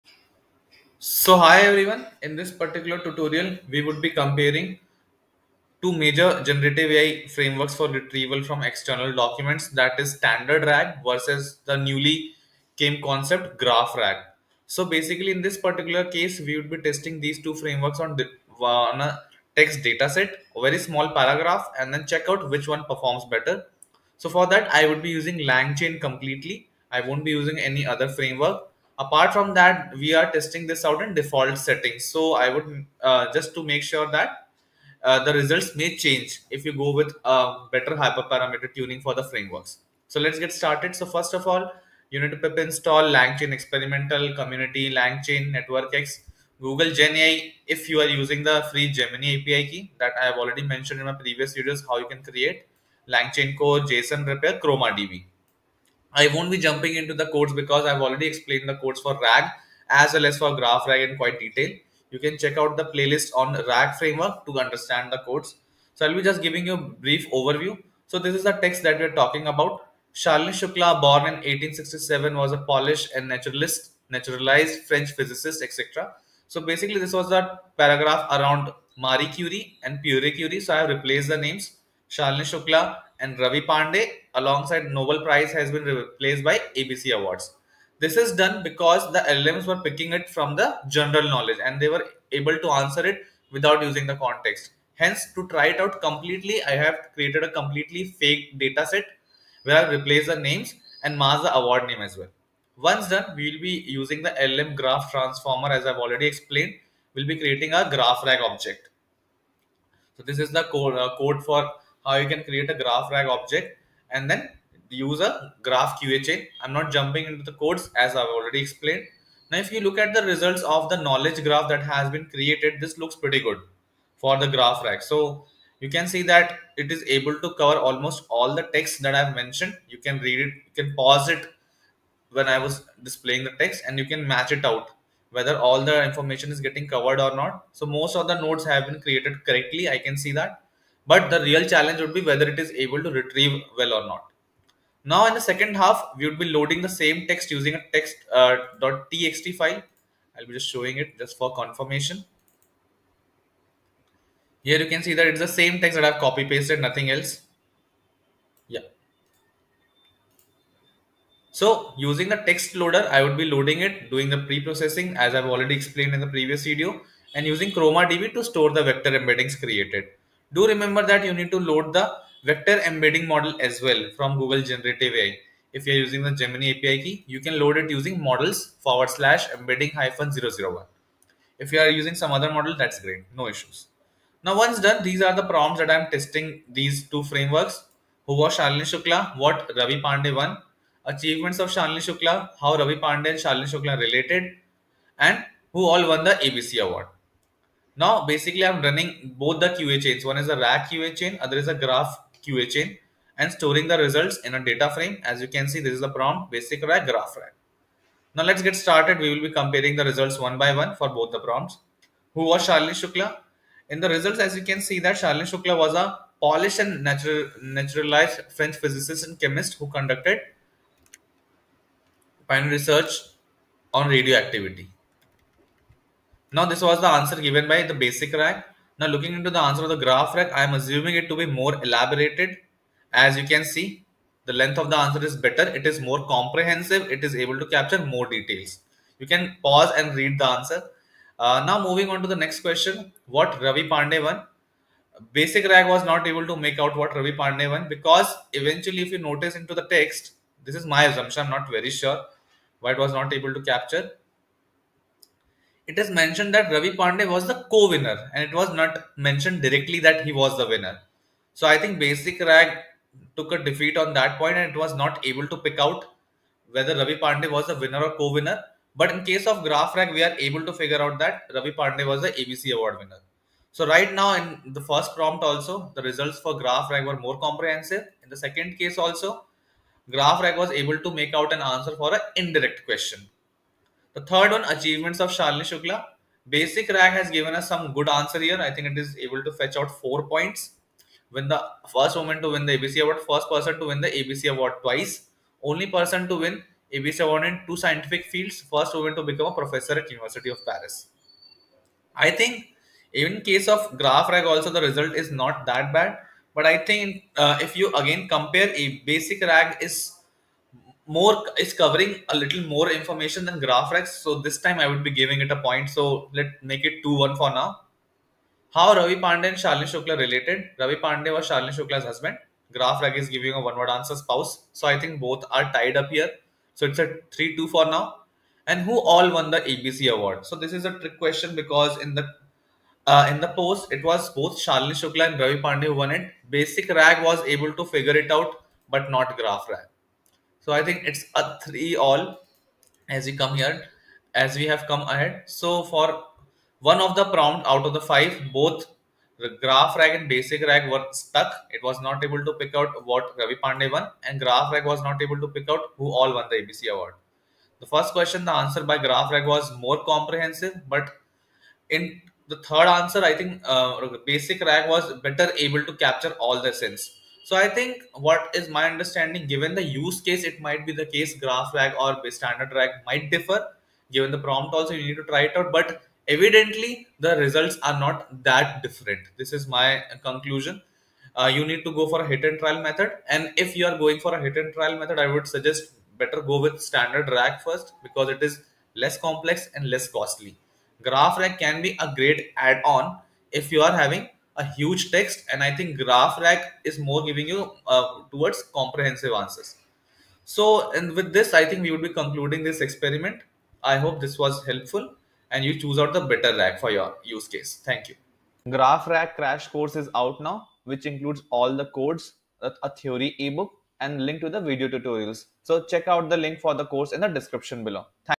GraphRAG vs RAG : Which is better? code comparison
This video compares GraphRAG vs standard RAG over text dataset and set of prompts to check which RAG performs better. Find all the codes below